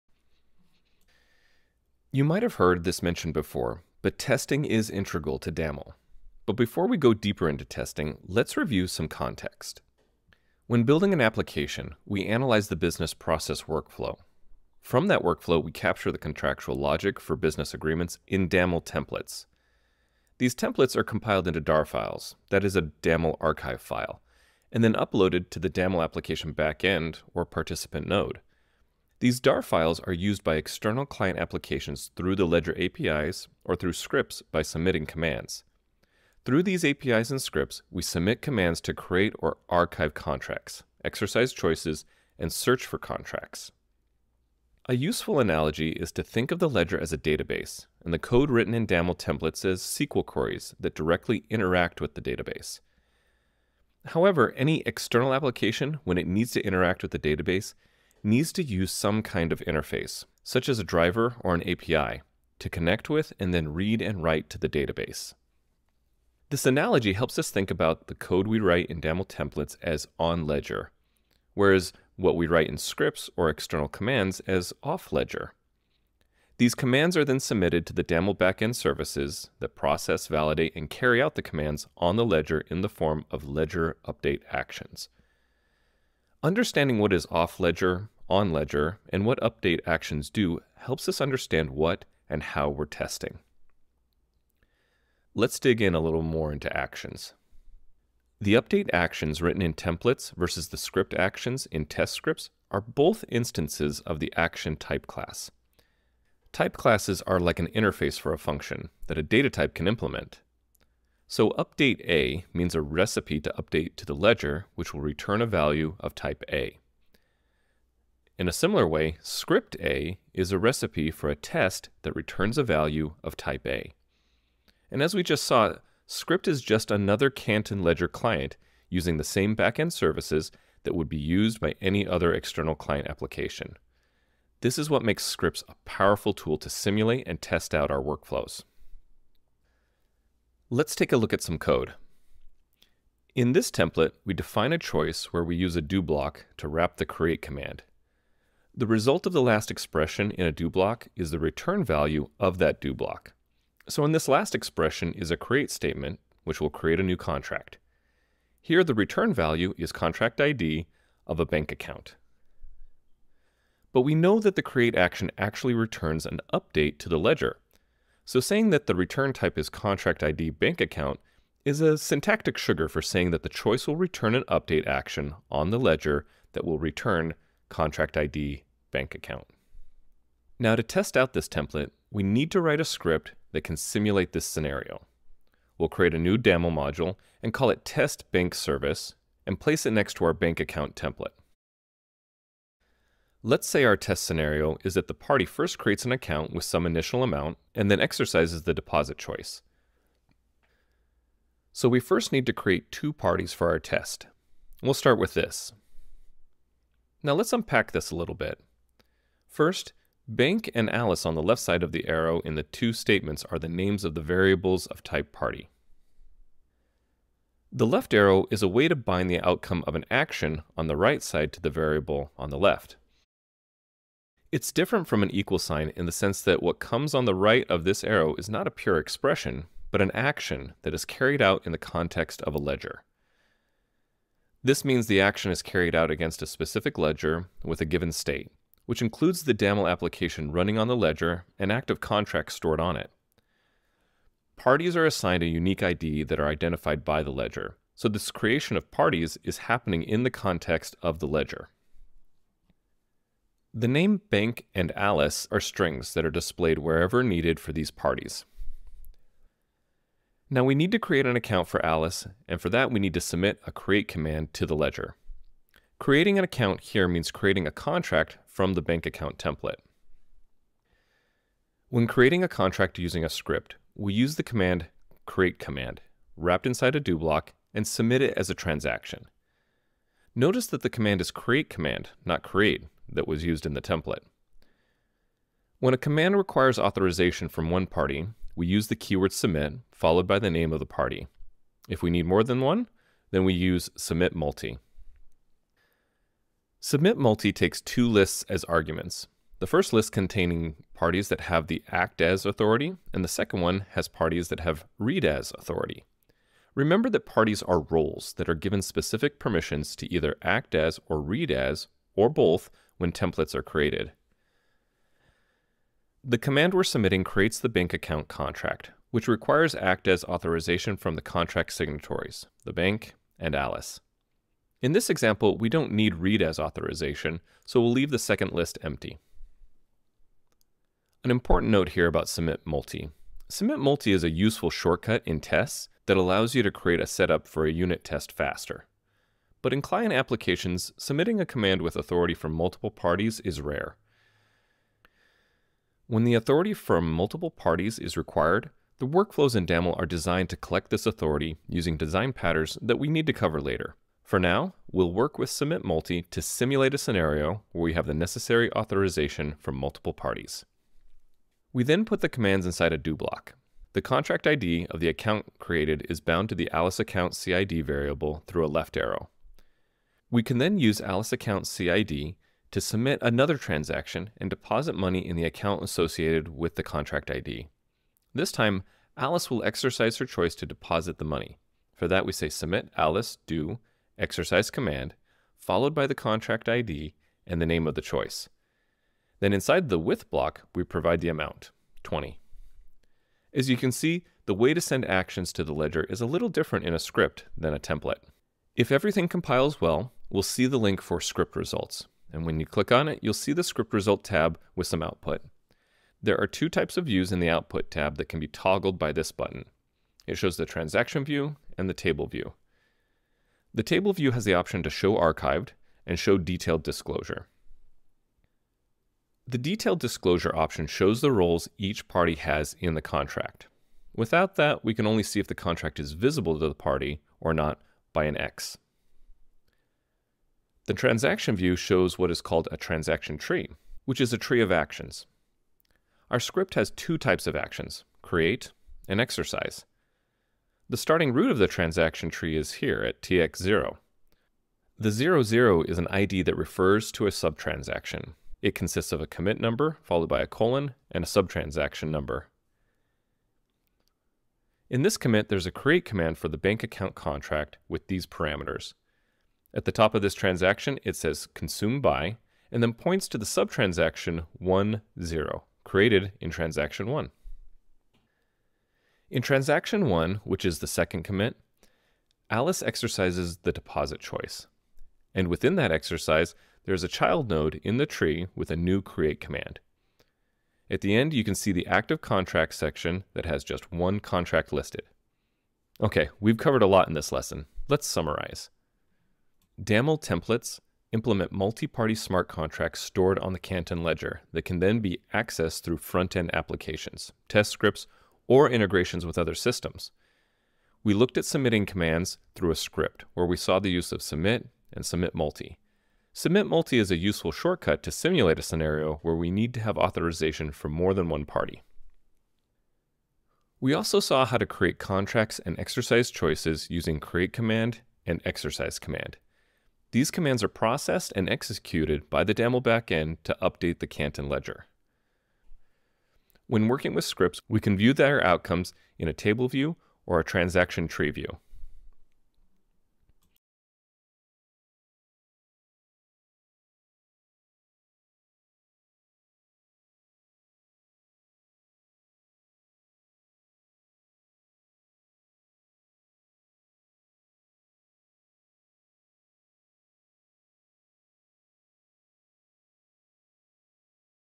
Lesson 4: Testing a Template
Daml templates implement multi-party smart contracts stored on the Canton Ledger that can then be accessed through front-end applications, test scripts, or integrations with other systems. We submit commands through scripts.

Submit Multi is a useful shortcut to simulate a scenario where we need to have authorization for more than one party. We can create contracts and exercise choices using create command and exercise command. These commands are processed and executed by the Daml backend to update the Canton Ledger.

When working with scripts, we can view their outcomes in a table view or a transaction tree view.

00:10 Daml and Canton
01:35 Actions on the ledger
02:20 Example code
03:00 Writing a test script
03:21 Parties
04:20 Create command
05:31 SubmitMulti
06:33 Running a test script
08:05 Review

For a better learning experience, sign up and enroll in this free course